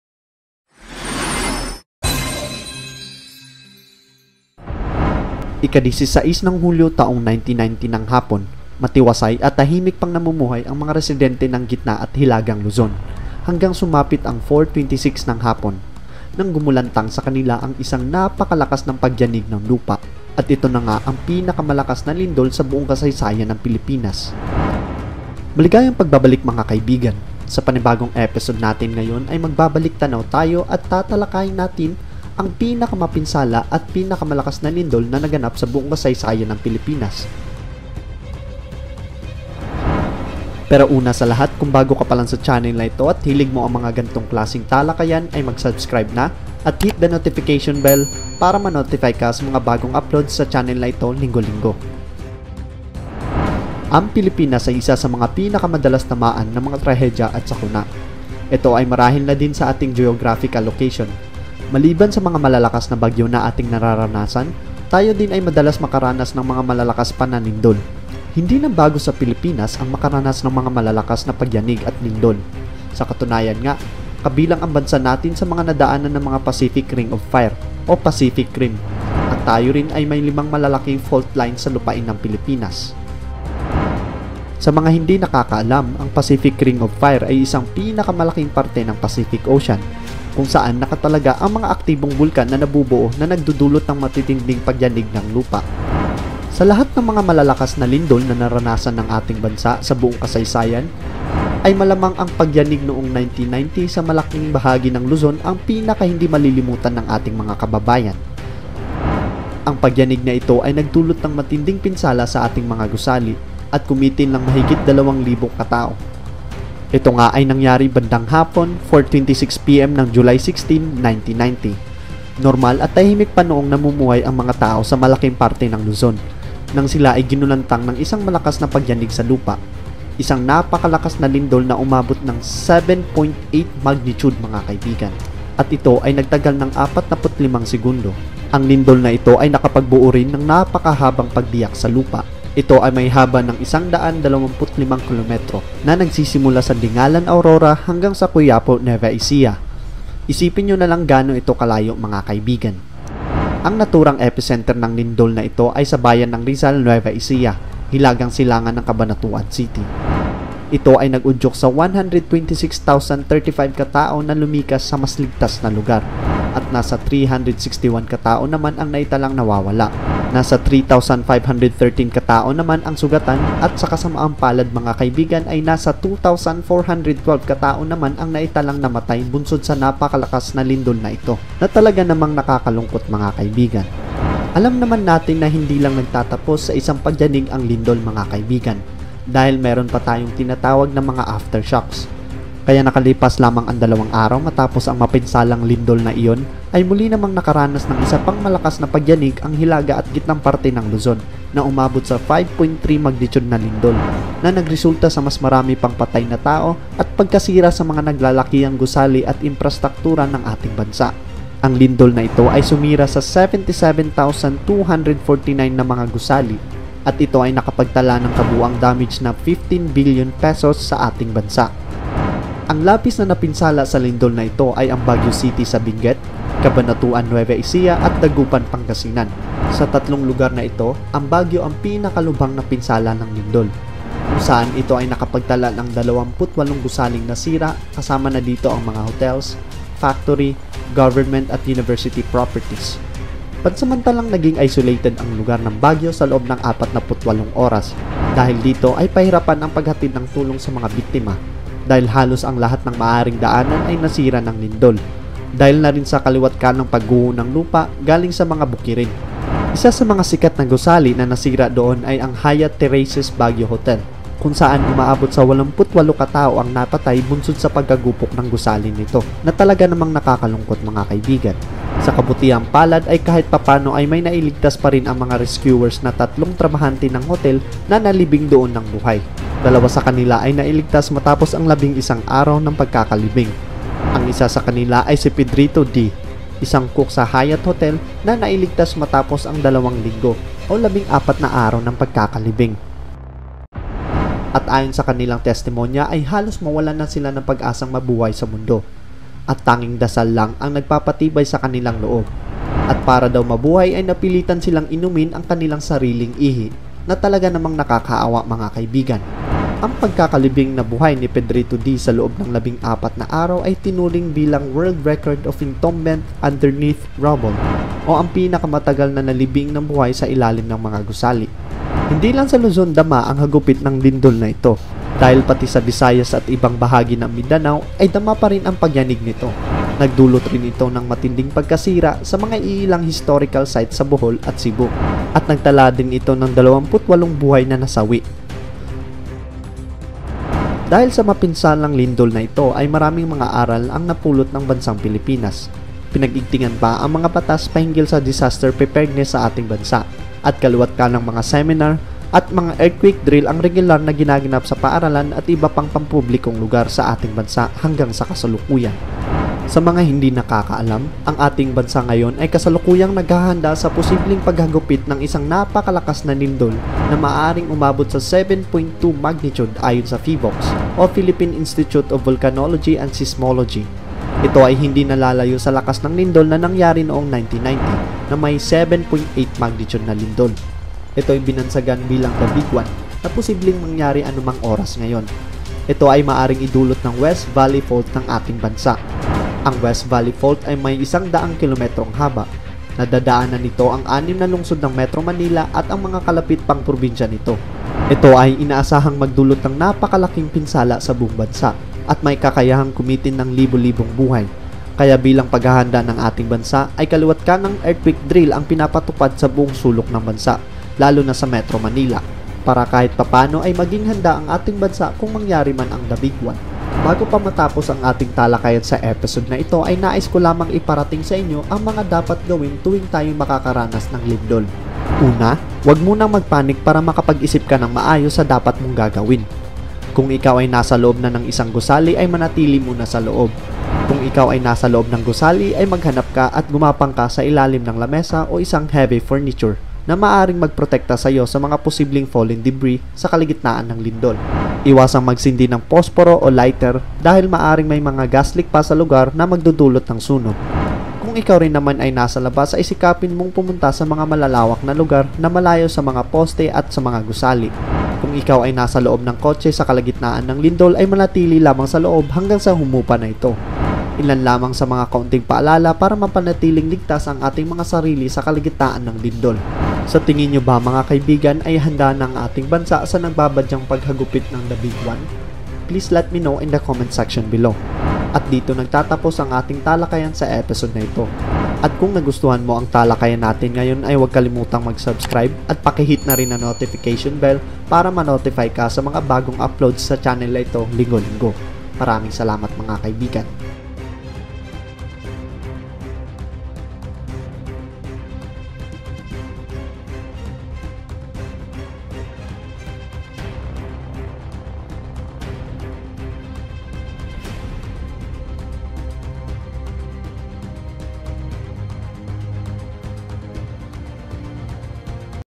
1990 LINDOL NA TUMANGAY NG MAHIGIT 2,000+ BUHAY SA PILIPINAS | TALAKAYAN PH
Pagbabalik tanaw sa LINDOL na tumama sa PILIPINAS taong 1990. Na kumitil ng mahigit dalawang libong buhay.

Ang Channel na ito ay nag tatampok ng mga kakaibang Talakayan na maari mo kapulutan ng aral.